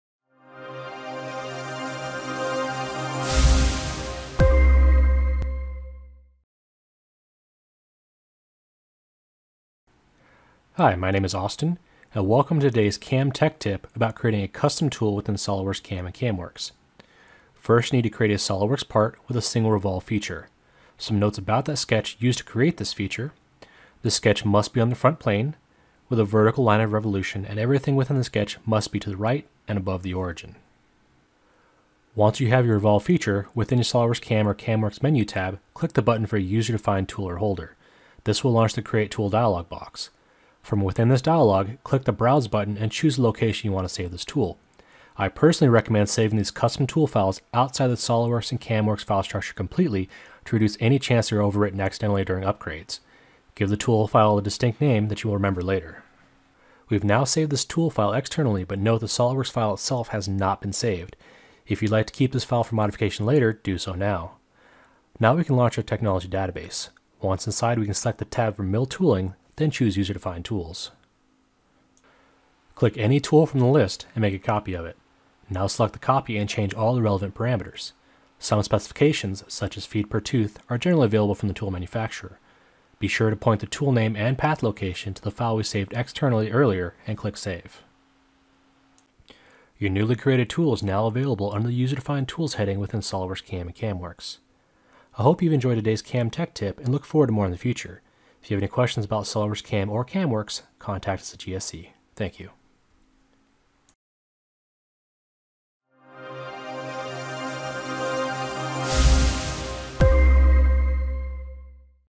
Creating a Custom Tool Within SOLIDWORKS CAM or CAMWorks
Learn how to create a custom tool within either @solidworks CAM or @CAMWorksTube. You will start with a SOLIDWORKS part with a single revolve feature.

The sketch used to create this feature has some parameters. The sketch must be on the front plane with a vertical line of revolution and everything within the sketch must be to the right and above the origin.

Go to your respective Menu Tab, and click the button for the User Defined Tool / Holder.

This will launch the Create Tool Dialogue Box. Save your tool file; our engineer recommends you save in a separate file structure. Use a distinct name for your recollection.

(Need to learn more with real-time instruction? Attend one of our online classes

In time you'll launch the technology database. Copy a tool from the list.

And you will find more detail in the tutorial! There are more SOLIDWORKStutorials on our blog as well